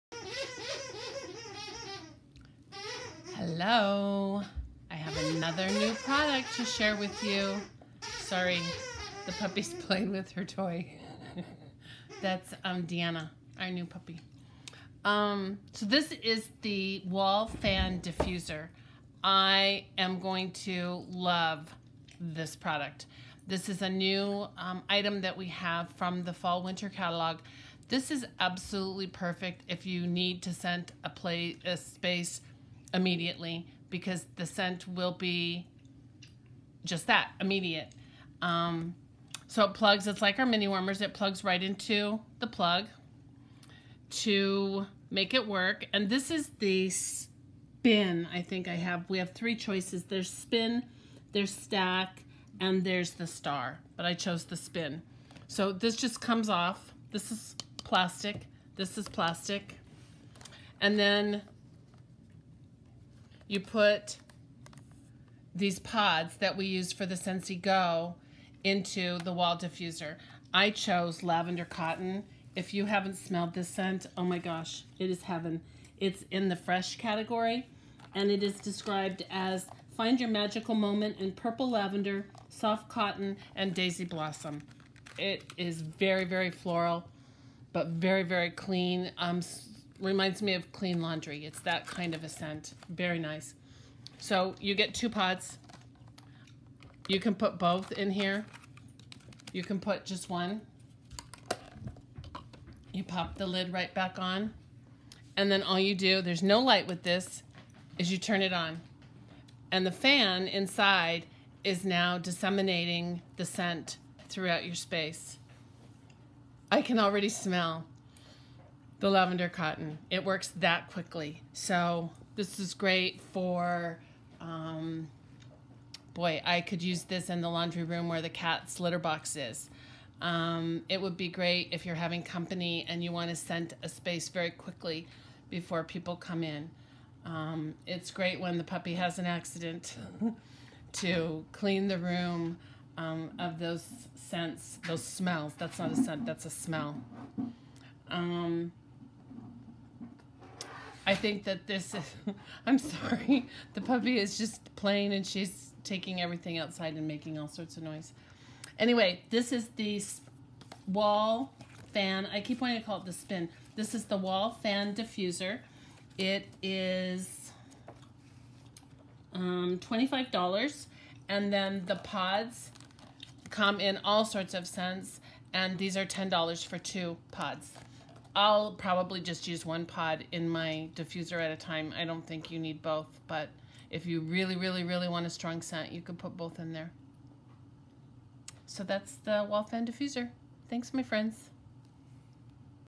Scentsy Wall Fan Diffuser
Plugs into any outlet and features a cute, sturdy shell that’s perfect for high-traffic areas.  Just add your favorite Scentsy Pod fragrance and you’re set.